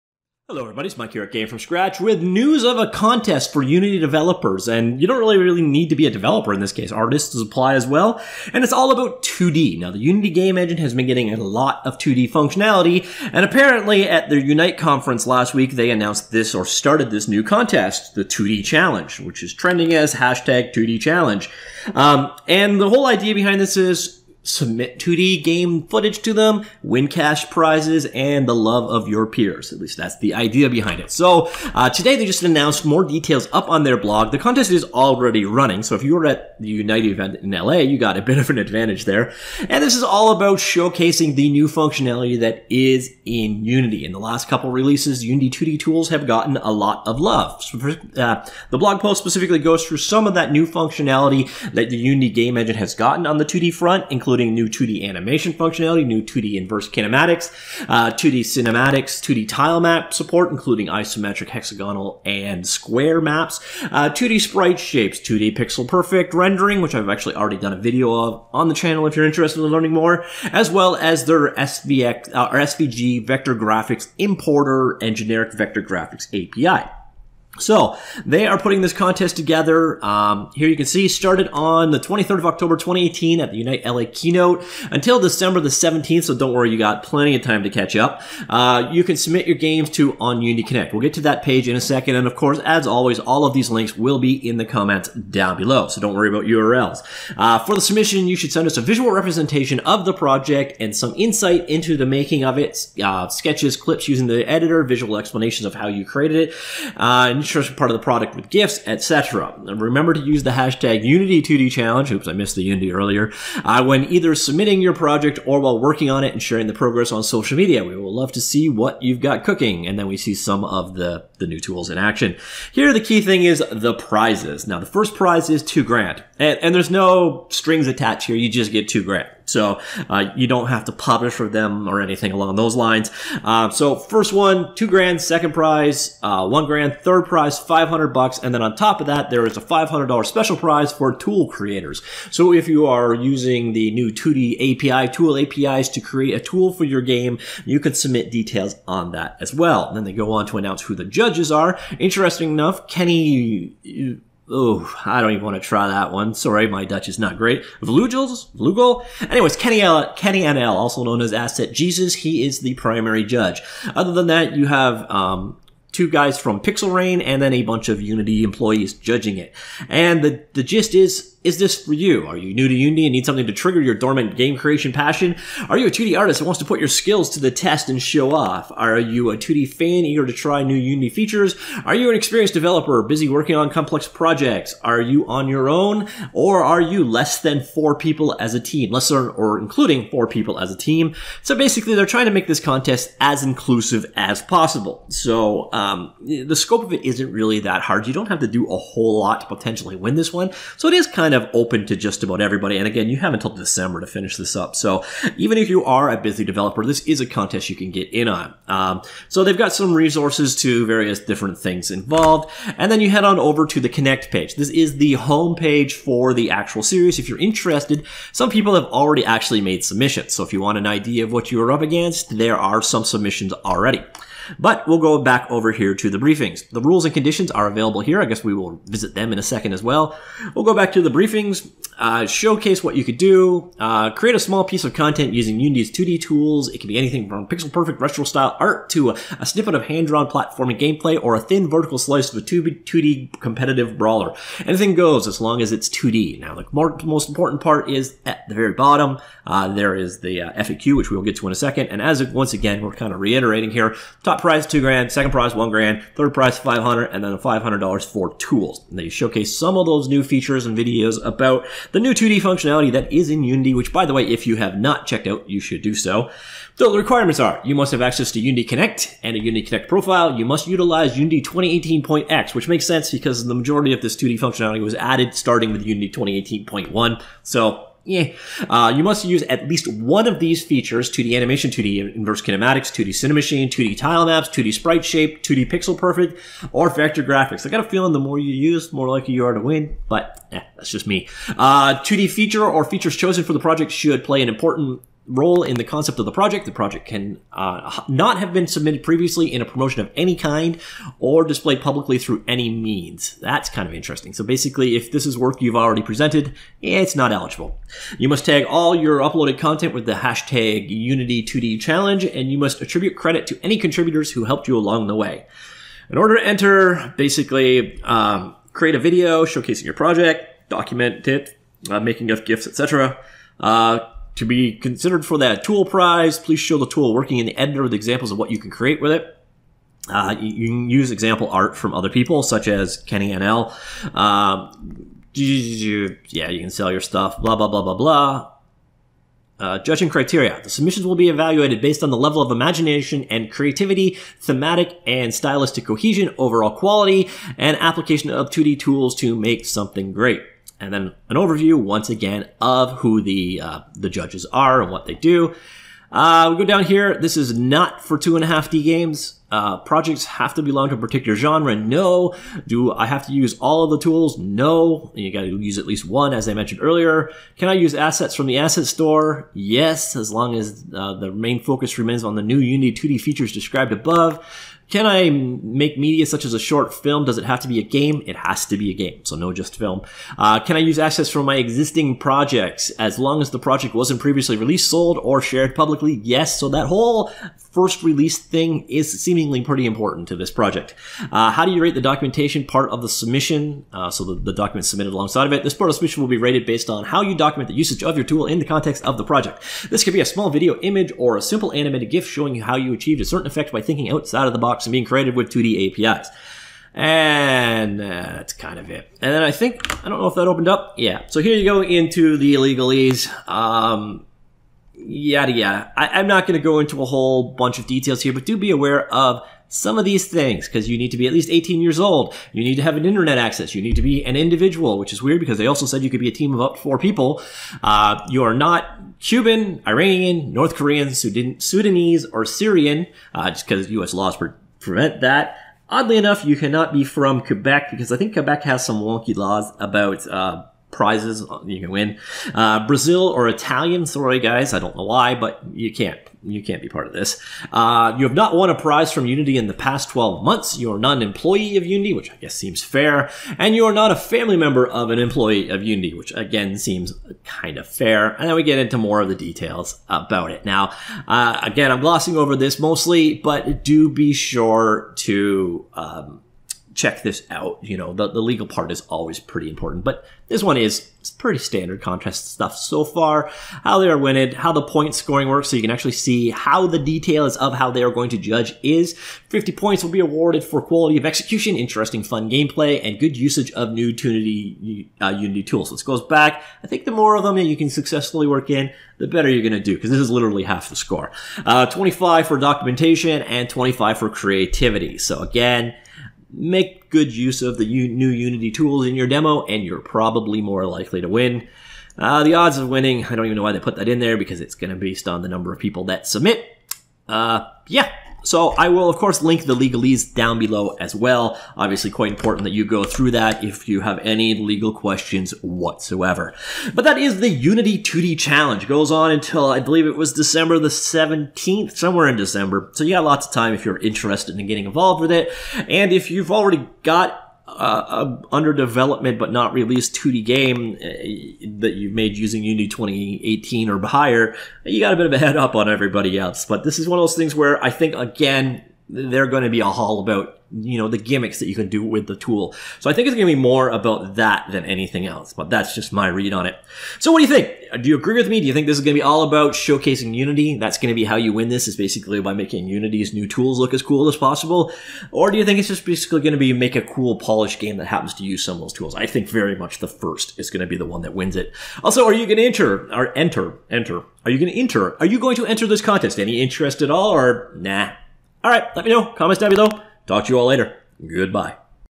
Unity 2D Challenge
Unity are currently running a contest, the Unity 2D Challenge, where you can win a first prize of $2000, second prize of $1000, third prize of $500 or a tools prize also of $500.

We all like cash, no?  You need to use Unity 2018 or higher and showcase one of the new features in Unity such as animation, IK, tilemaps, Cinemachine, Pixel Perfect or SpriteShape.  Contest ends mid December, 2018.